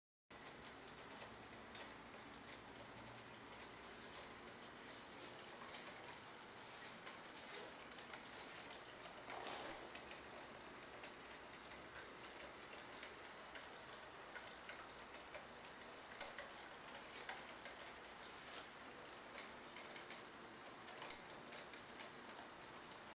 Horta vertical hidropônica, primeira tentativa
Acho que está pingando muita água, semana que vem pretendo colocar um registro na ponta